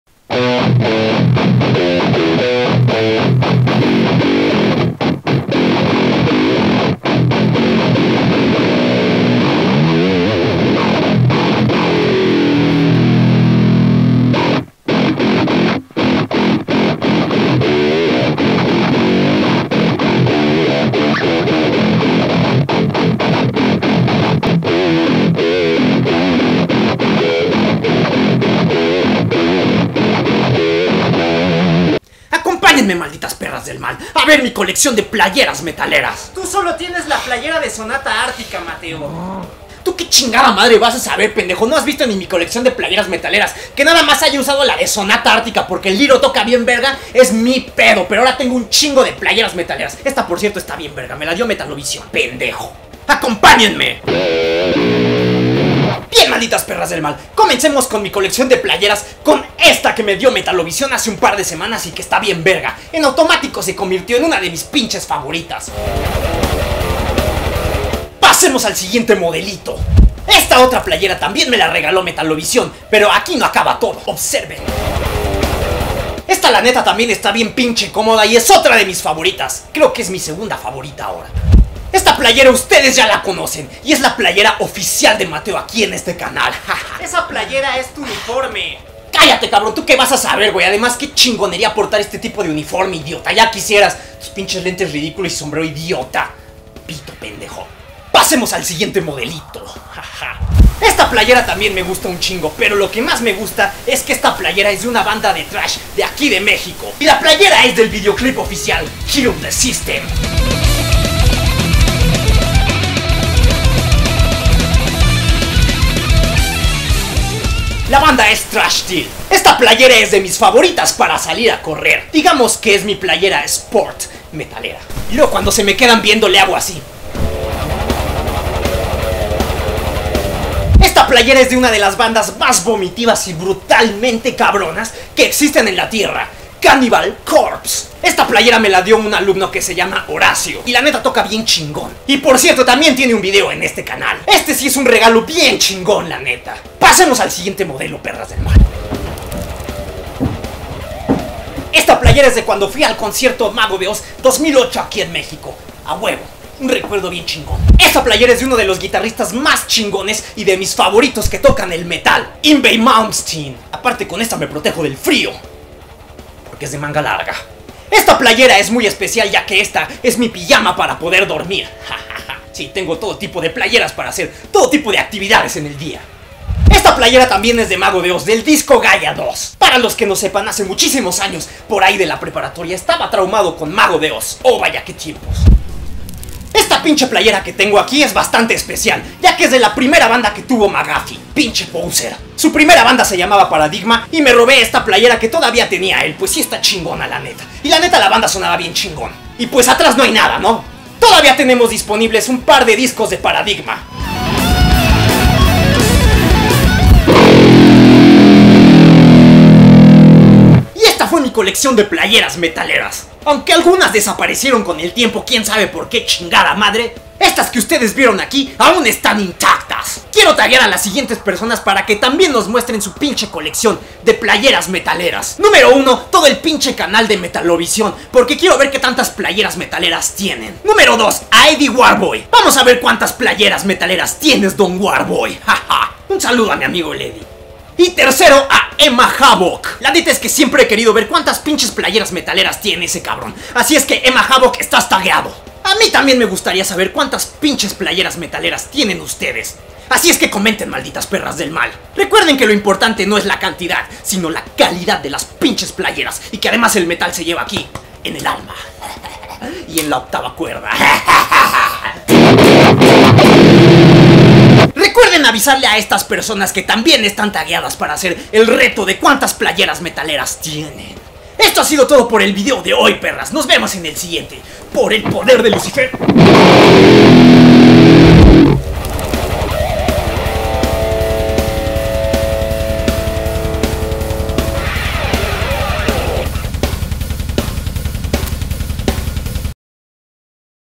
TAG de las playeras METALERAS - Mateo
Te gusta el metal, bien, pero, ¿cuántas playeras o camisetas tienes?

¿Gustas contactarme? ¿Te gustarían clases de guitarras 100% personalizadas?